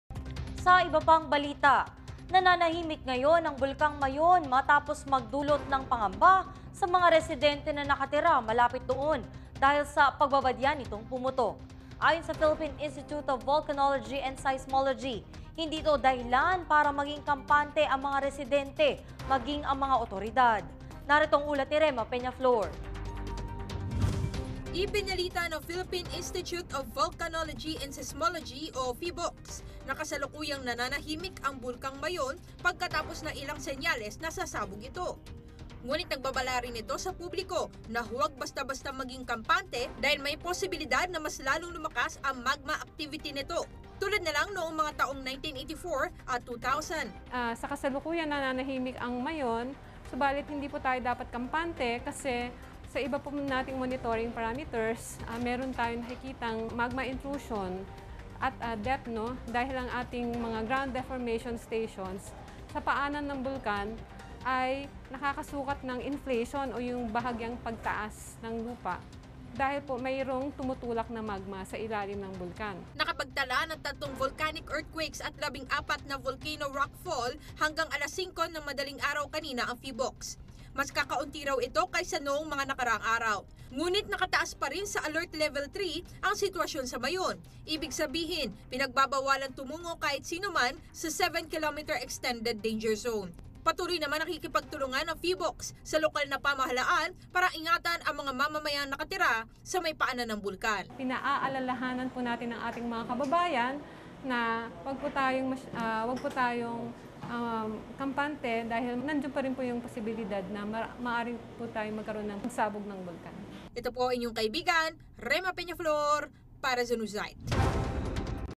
Mayon Volcano update as of September 22
PHIVOLCS, nagbabala sa publiko na huwag maging kampante sa kasalukuyang pananahimik ng bulkang Mayon

September 22, 2014 by Rhema Peñaflor